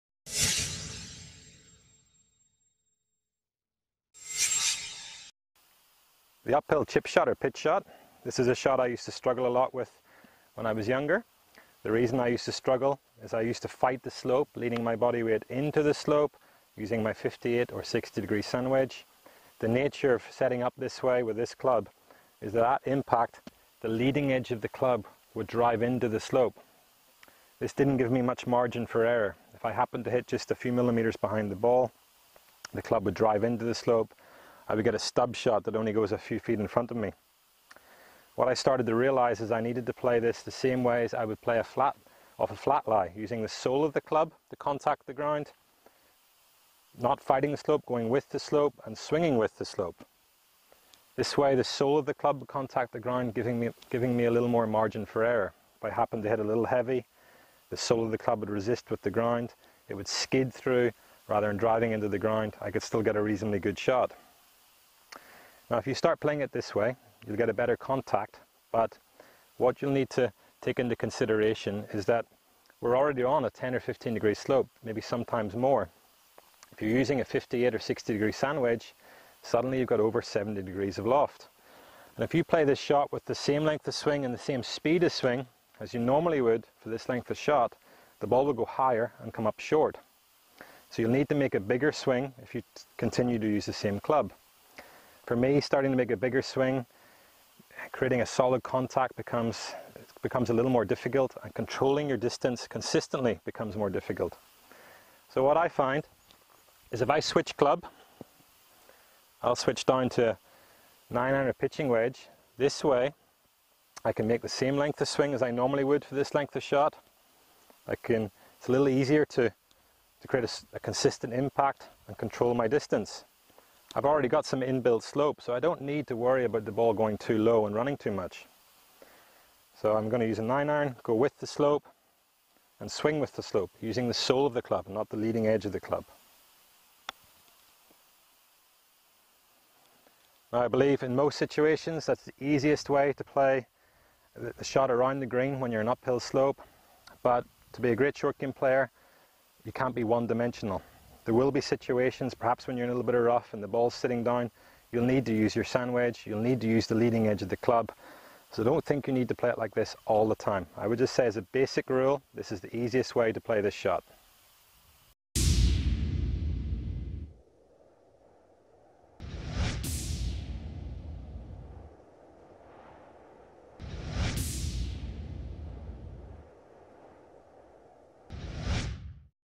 UPHILL LIE CHIP SHOT
CHIPPING FROM AN UPHILL LIE - PGA Professional Robin Symes explains how to play a chip or pitch when your are on a uphill lie approaching the green.
Transcript
The Uphill Chip Shot or Pitch shot. This is the shot I used to struggle a lot with when I was younger. The reason I used to struggle is I used to fight the slope leaning my body weight into the slope using my 58 or 60 degree sand wedge. The nature of setting up this way, with this club, is that at impact, the leading edge of the club will drive into the slope. This didn't give me much margin for error. If I happened to hit a few millimeters behind the ball, the club would drive into the slope, I would get a stub shot that only goes a few feet in front of me.
What I started to realize is that I needed to play this the same ways I would play a flat, off a flat lie, using the sole of the club to contact the ground, not fighting the slope, going with the slope, and swinging with the slope. This way the sole of the club will contact the ground, giving me a little more margin for error, if I happen to hit a little heavy, the sole of the club would resist with the ground. It would skid through rather than driving into the ground. I could still get a reasonably good shot.
Now if you start playing it this way, you will get a better contact, but what you'll need to take into consideration is that, we're already on a 10 to 15 degree slope, maybe sometimes more. If you are using a 58 or 60 degree sand wedge, suddenly you've got over 70 degrees of loft. And if you play this shot with the same length of swing and the same speed of swing as you normally would, for this length of shot, the ball will go higher and come up short. So you'll need to make a bigger swing if you continue to use the same club.
For me, starting to make a bigger swing, creating a solid contact, becomes a little more difficult. And controlling your distance consistently becomes more difficult. So what I find, is if I switch club, I'll switch down to 9 iron pitching wedge, this way I can make the same length of swing as I normally would to this length of shot. It's a little easier to create a consistent impact and control my distance. I've already got some in-built slope, so I don't need to worry about the ball going too low and running too much. So I'm going to use a nine iron, go with the slope, and swing with the slope, using the sole of the club, not the leading edge of the club.
I believe in most situations, that's the easiest way to play the shot around the green, when you're in uphill slope. But to be a great short game player, you can't be one dimensional. There will be situations perhaps when you're in a little bit of rough and the ball sitting down. You will need to use your sand wedge, you will need to use the leading edge of the club, so don't think you need to play it like this all the time. I would just say, as a basic rule, this is the easiest way to play this shot.